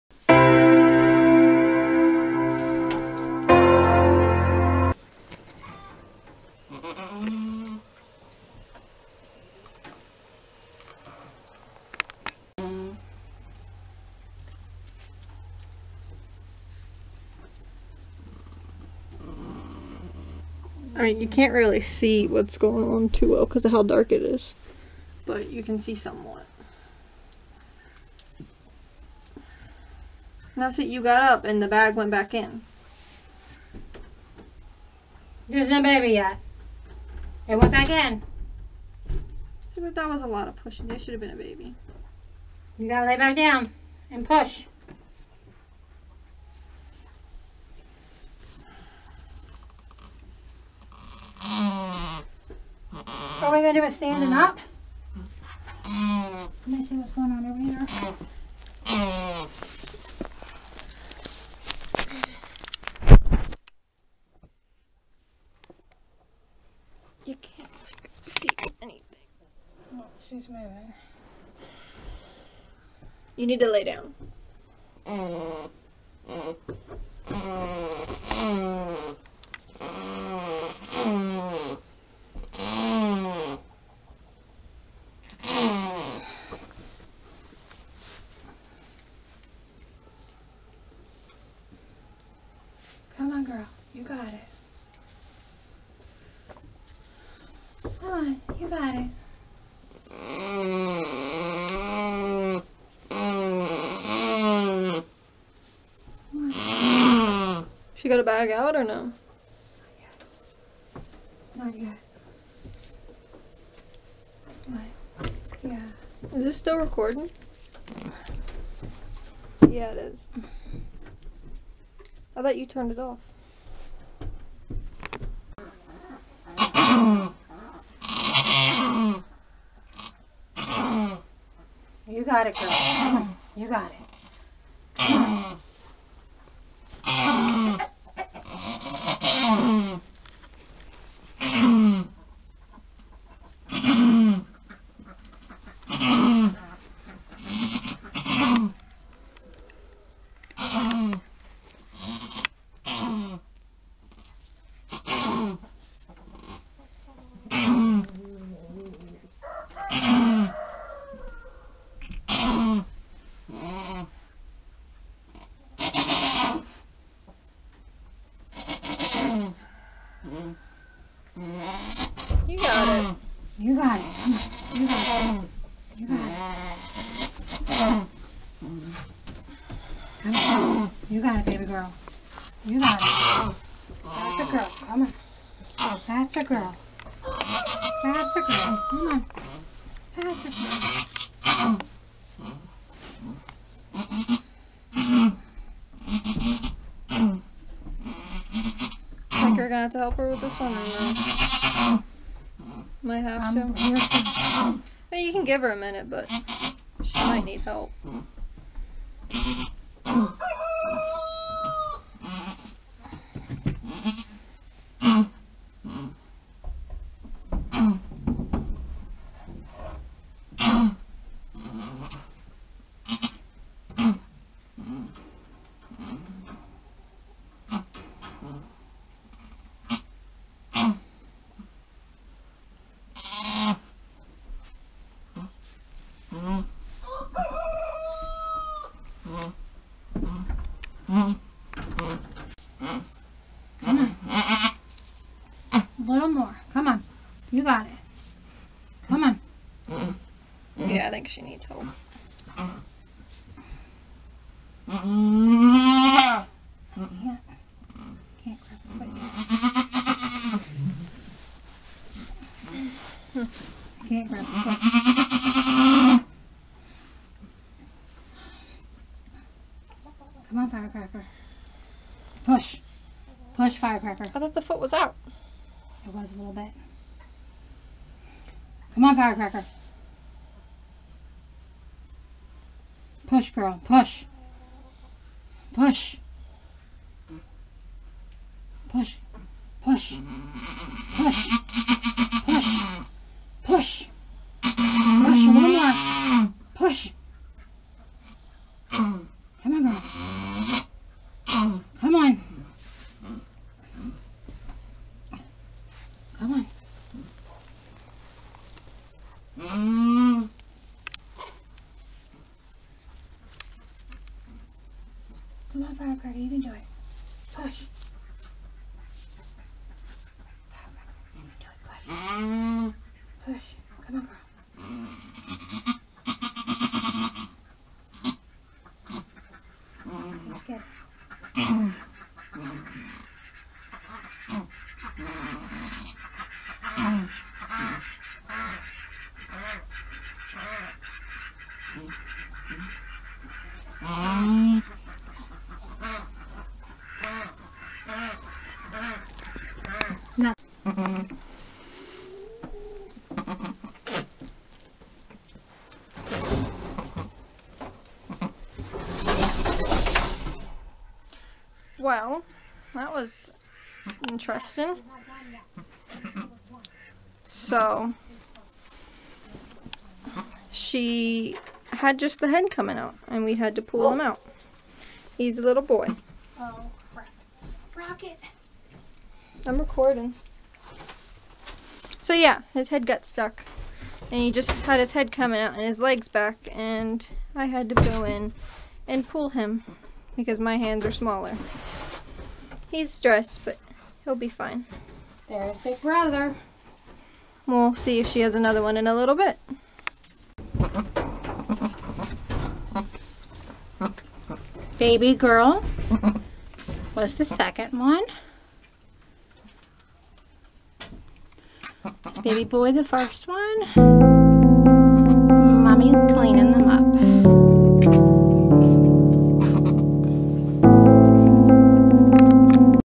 Firecracker Kidding Video
On March 15th Firecracker went into labor and had a set of twins buck/doe. Graham & Vanilla.